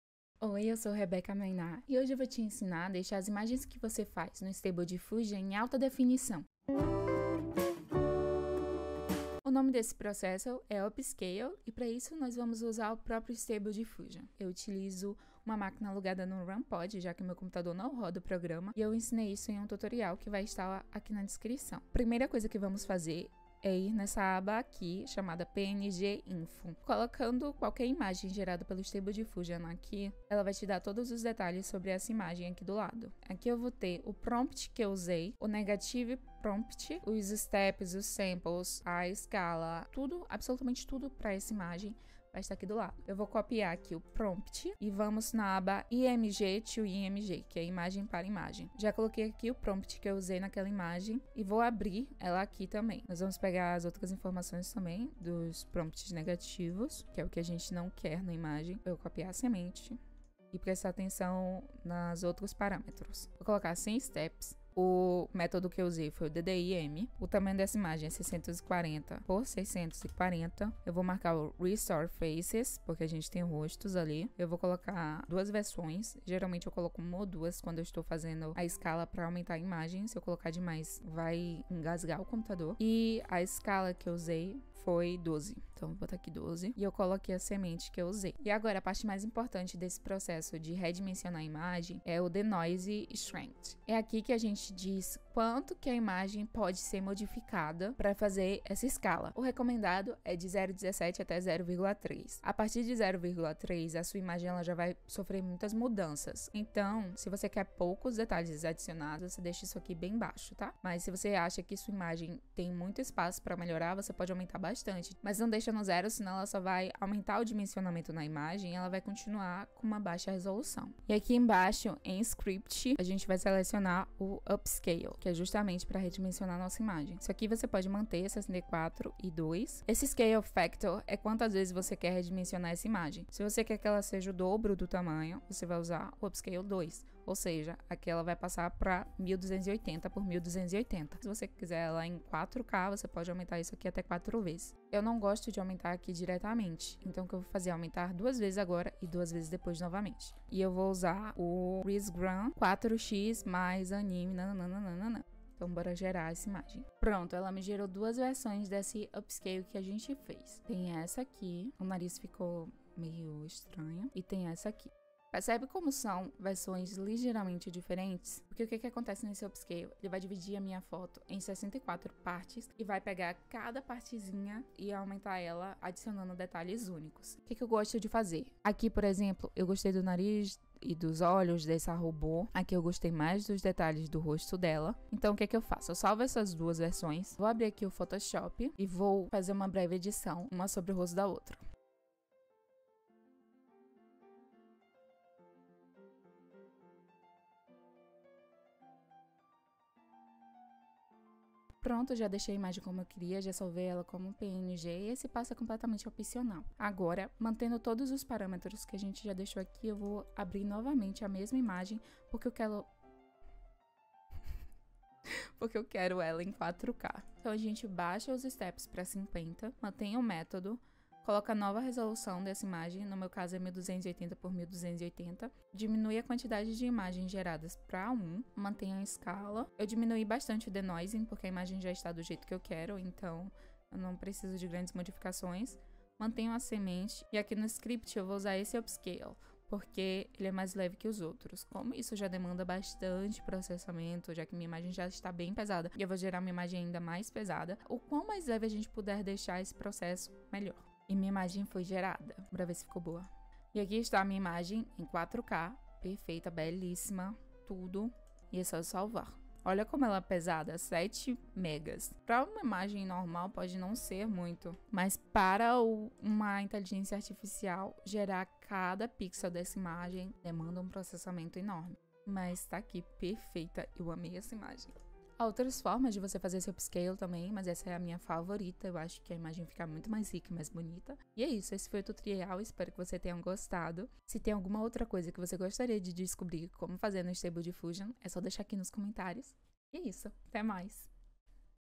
Suas imagens do Stable Diffusion em ALTA RESOLUÇÃO 4k (img2img)
Tutorial simples para redimensionar as imagens feitas pelo Stable Diffusion para alta definição, em 4k. Esse processo se chama upscale e é feito no próprio Stable Diffusion na aba img2img.

Há outras maneiras de fazer, mas essa é a minha favorita!

Capítulos:
00:00 - Intro
00:22 - Primeiro upscale no Stable Diffusion
02:57 - Photoshop (opcional)
03:21 - Segundo upscale para 4k